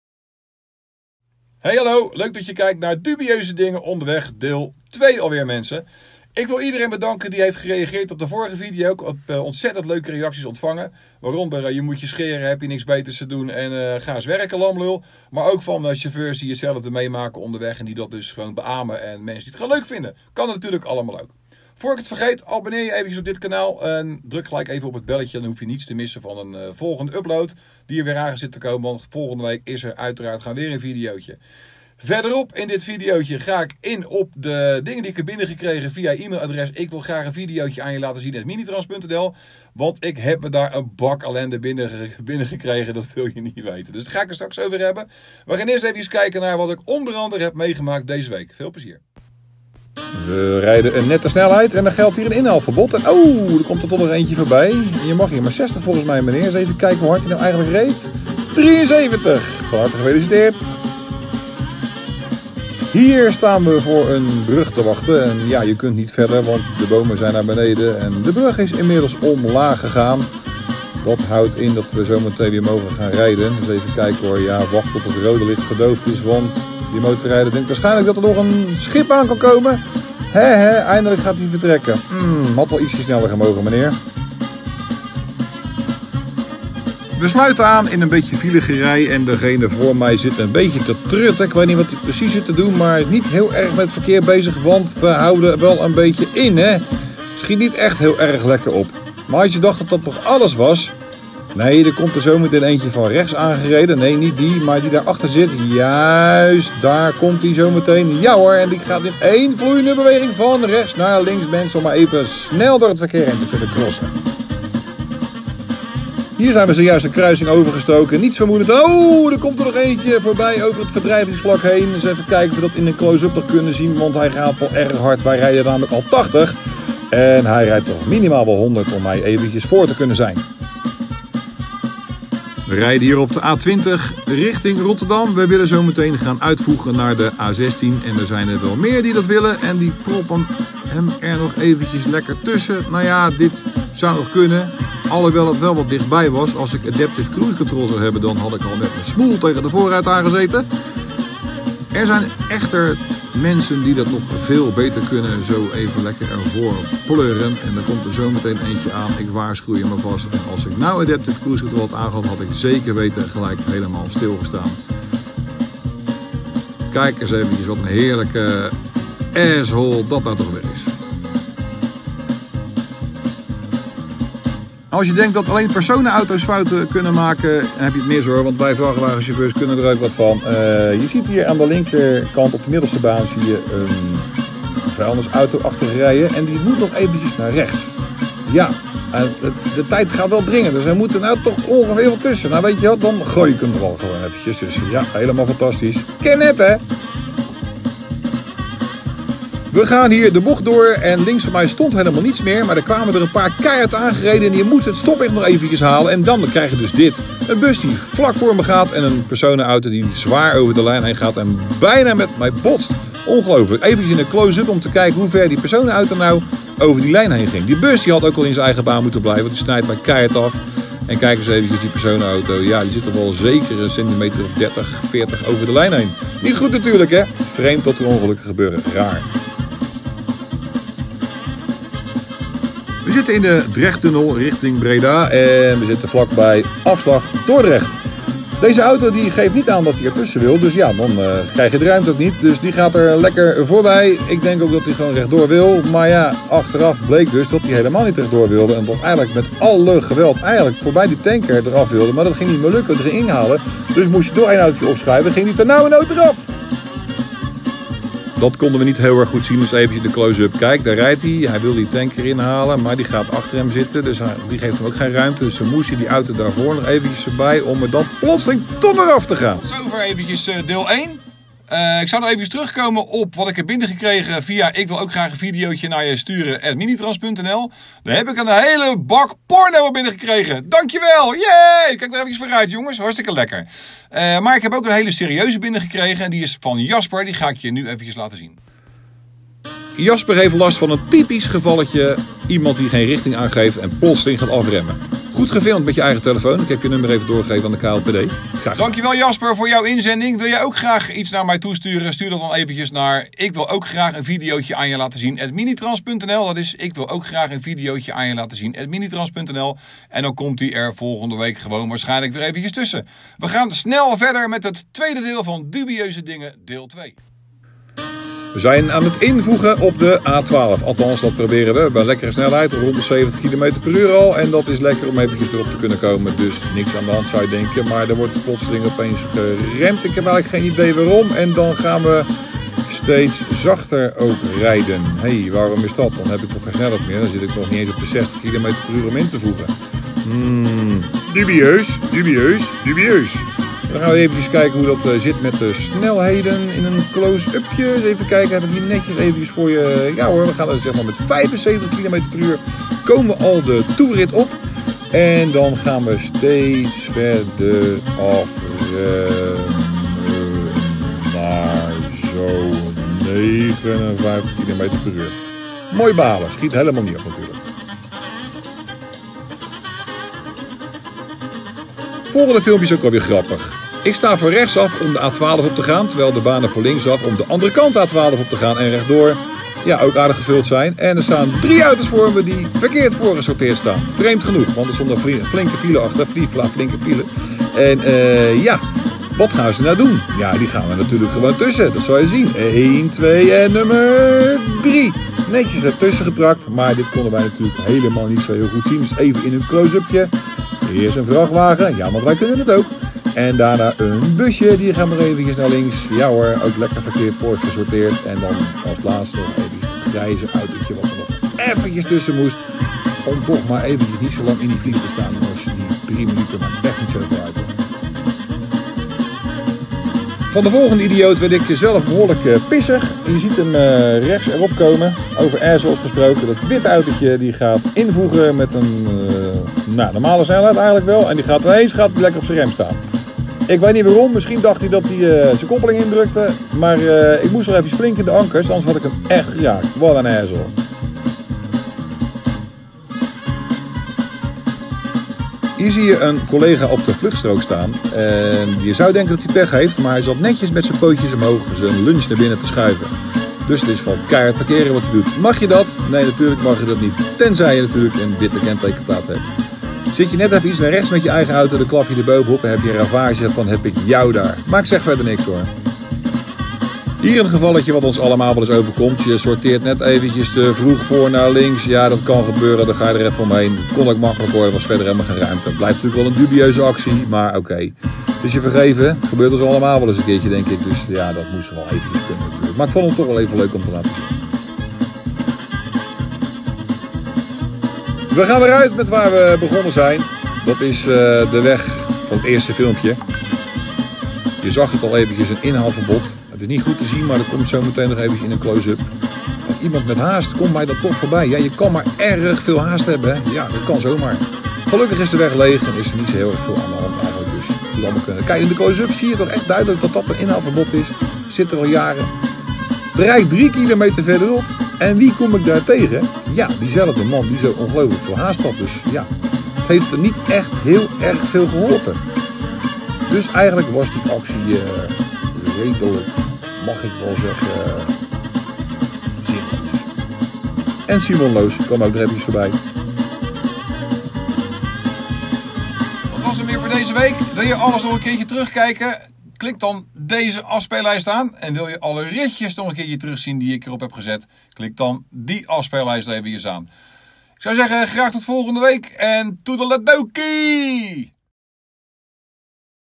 Truckin' Bart #25: Dubieuze Dingen Onderweg Deel 2
Dubieuze dingen, gefilmd met de dashcam. De acties in dit korte filmpje is van slechts 5 dagen, en dan laat ik niet eens alles zien!!
En dan hebben we het nog niet eens over wat er náást de auto gebeurd (geen camera's daar hè?).
Wil jij jouw filmpje laten zien; volg de instructies in deze video..